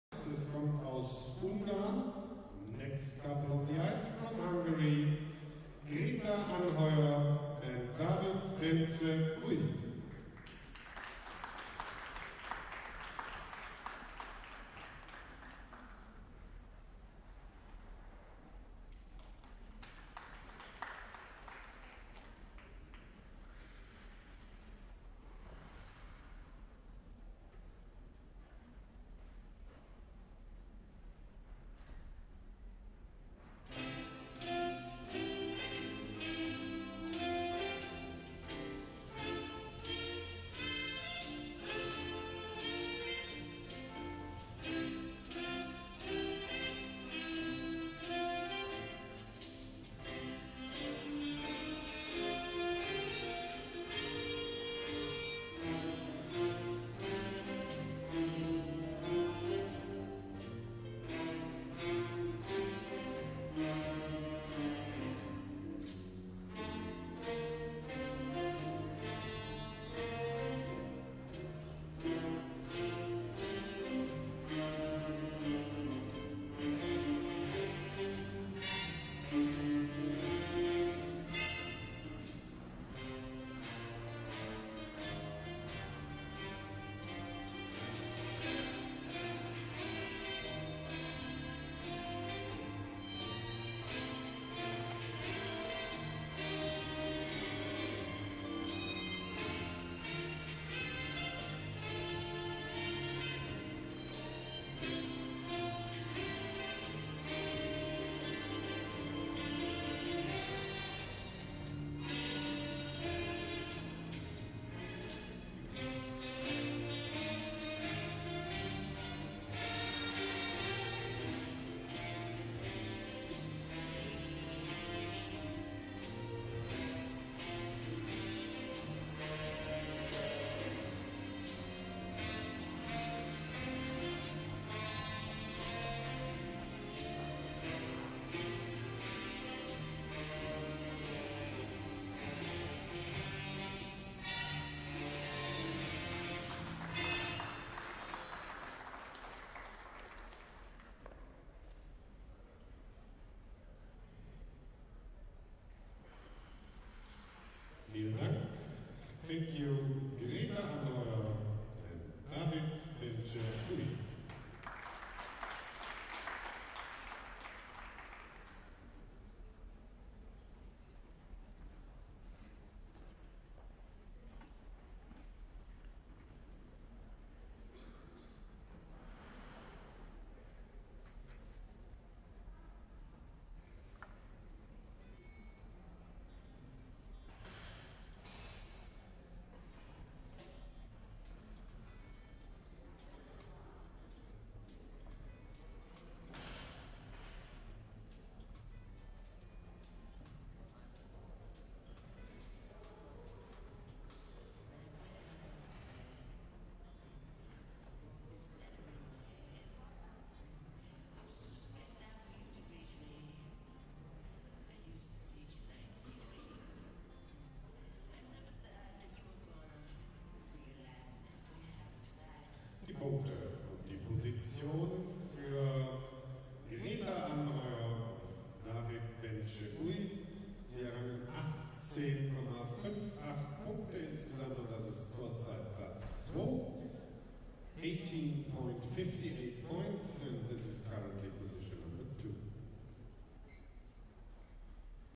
Greta ANHEUER / David Bence UJJ (HUN) Pattern Dance 2 @ NRW Trophy 2015
NRW Trophy for Ice Dance
Dortmund / GER
Advanced Novice - Pattern Dance 2 (With Keypoints)
Total Segment Score: 9.29